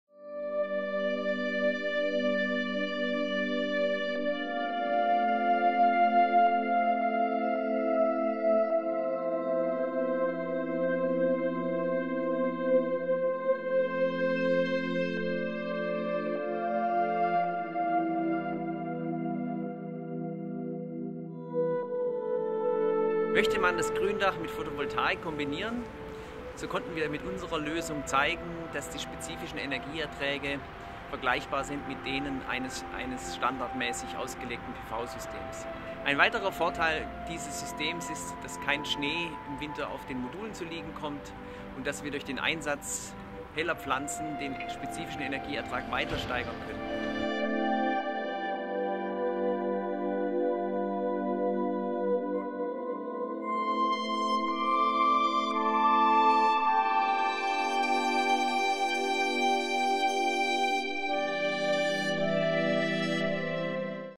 Senkrechte Solaranlagen und kühlende Gründächer sind eine ideale Ergänzung
Wegen des Klimawandels werden kühlende Gründächer immer wichtiger. Dank senkrechten Solarmodulen können Pflanzen auf Dächern gut neben Solaranlagen gedeihen. Gemäss ZHAW-Messungen sind die spezifischen Energieerträge mit denen herkömmlicher Solaranlagen vergleichbar. Zudem führt eine helle Dachbegrünung zu mehr Stromertrag.